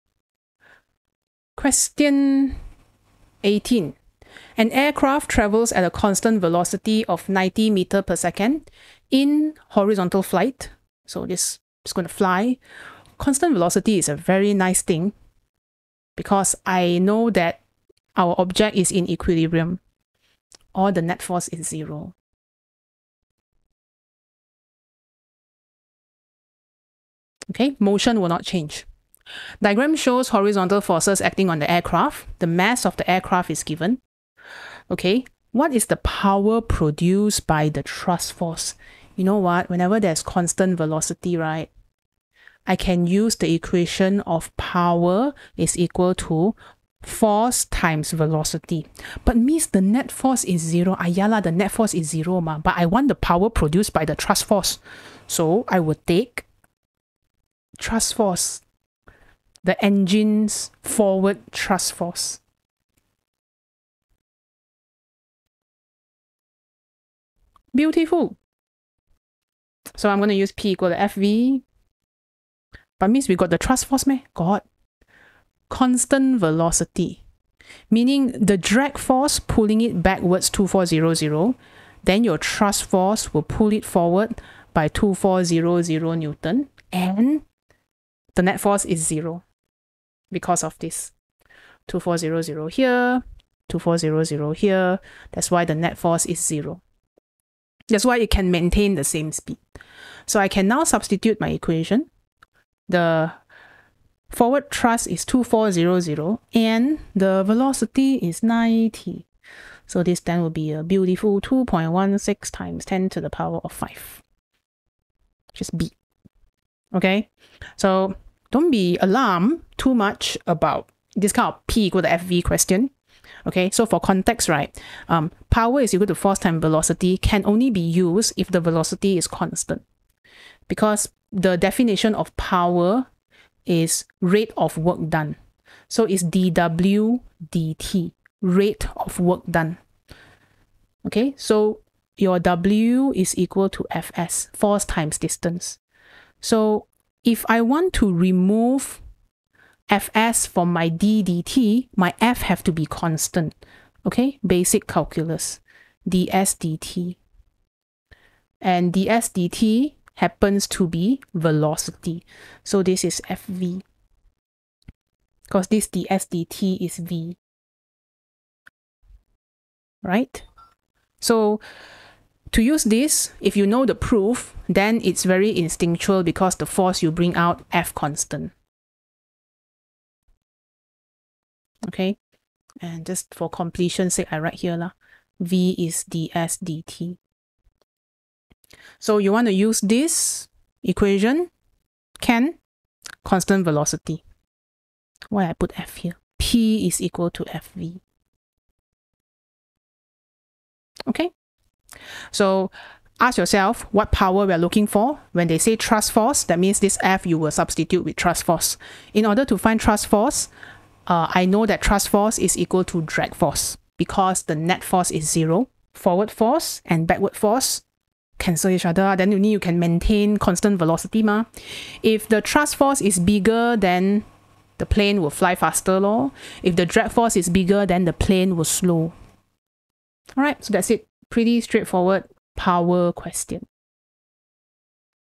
5.2a Ex1 FM21 P12 Q18 Power of Aircraft Thrust Force | AS WEP | Cambridge A Level 9702 Physics
Example 1 - 9702/12/F/M/21: An aircraft travels at a constant velocity of 90m s–1 in horizontal flight. The diagram shows the horizontal forces acting on the aircraft. The mass of the aircraft is 2000 kg. What is the power produced by the thrust force?

9702_m21_qp12